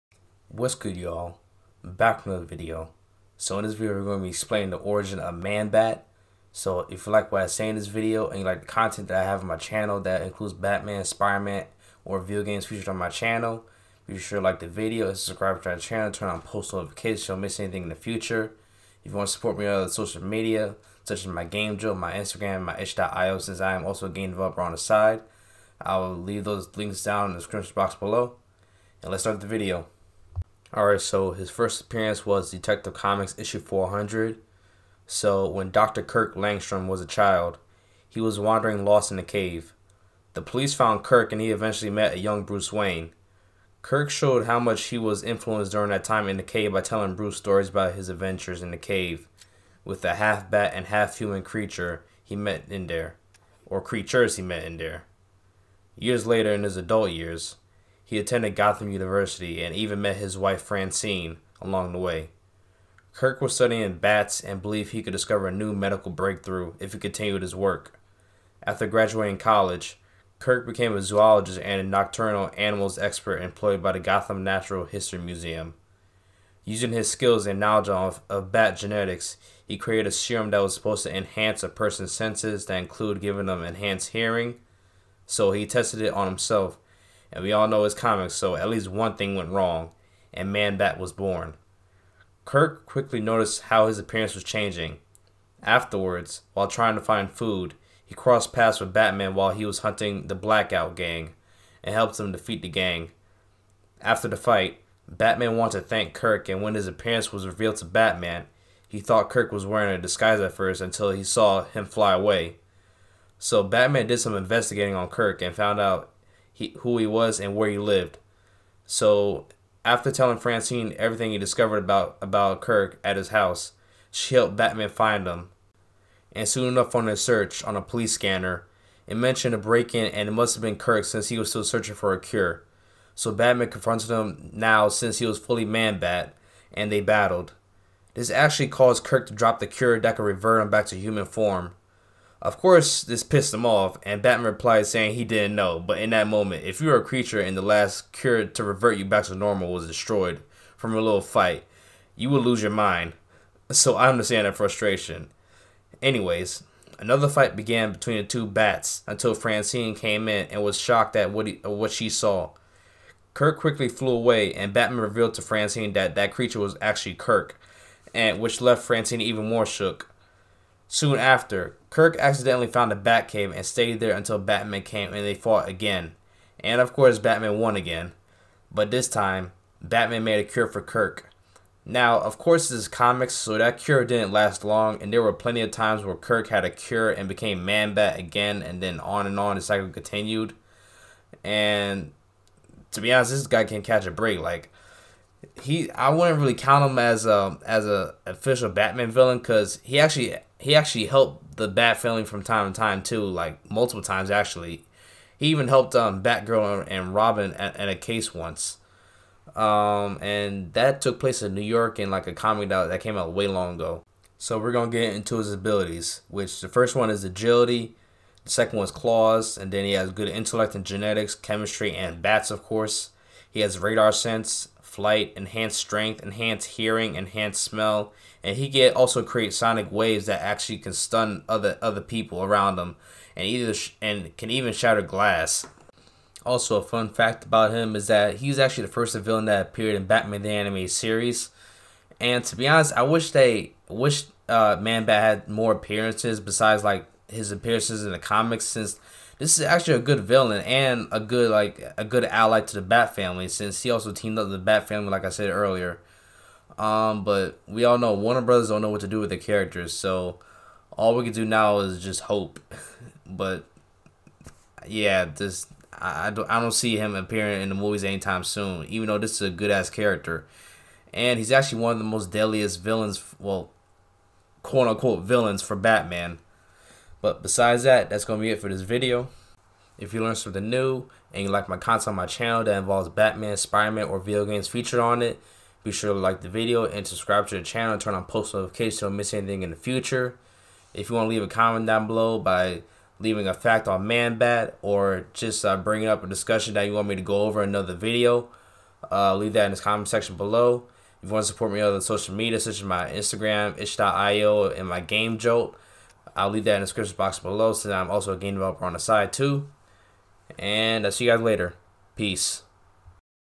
Giant Bat In Gotham That's Not Batman? (Origin Of Manbat)
In this video, we find out how Manbat was born